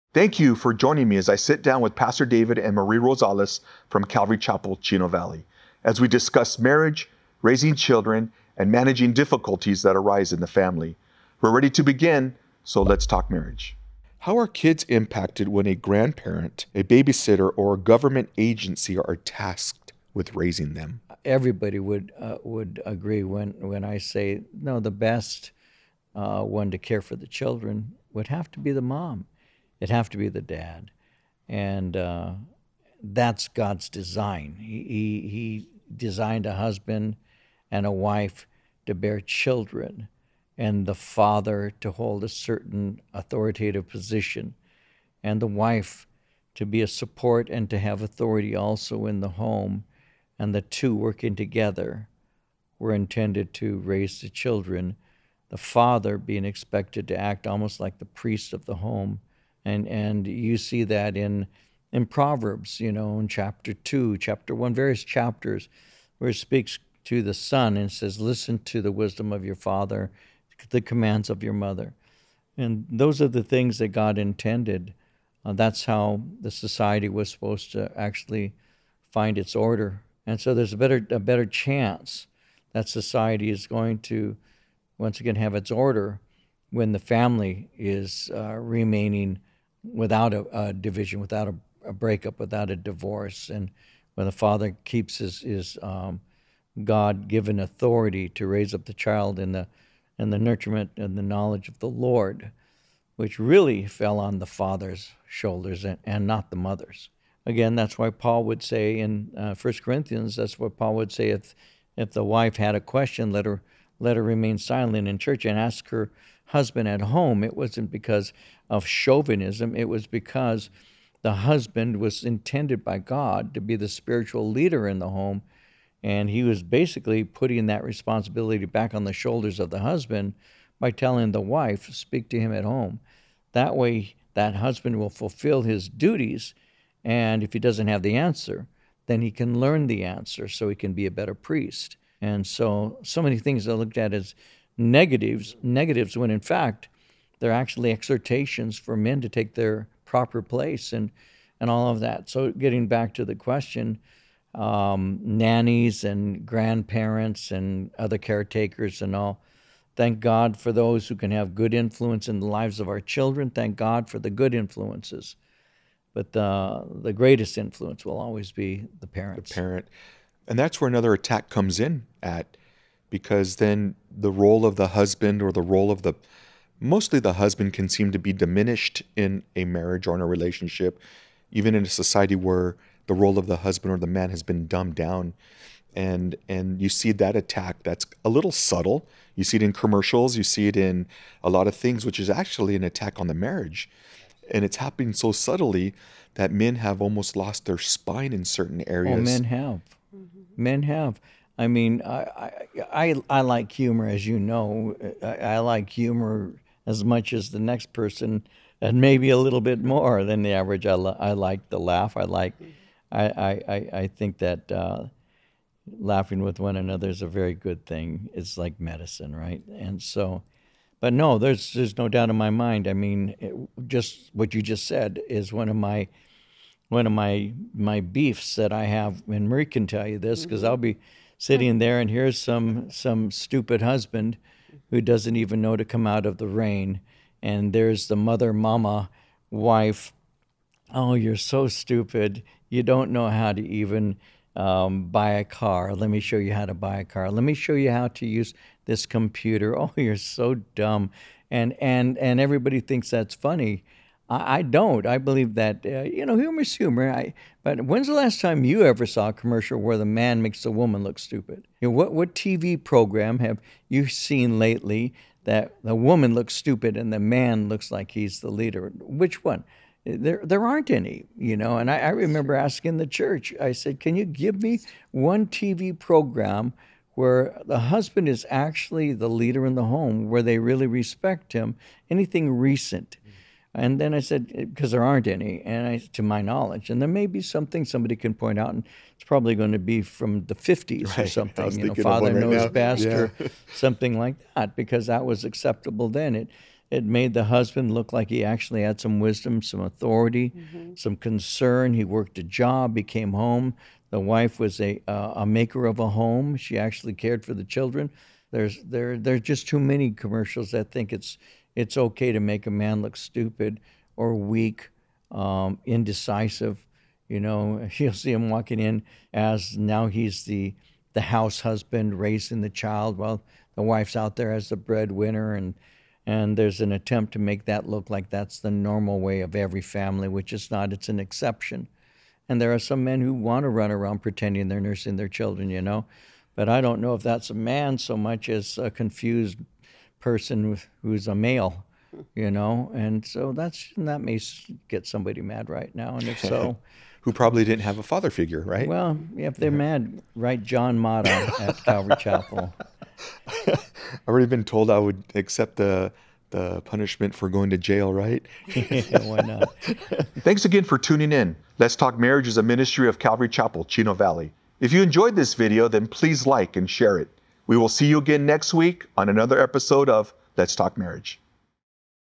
Culture's Effect On Husbands | Let's Talk Marriage
One of man's greatest responsibilities is establishing order and a godly influence in the home. In this episode of 'Let's Talk Marriage,' Pastor David emphasizes the need for men to stand up and fulfill their callings as husbands and fathers. Men, the task at hand can be hard to bear, but seeking the Lord for instruction will strengthen you to lead your family in the faith. We hope you are challenged and encouraged by this timely conversation on 'Let's Talk Marriage.'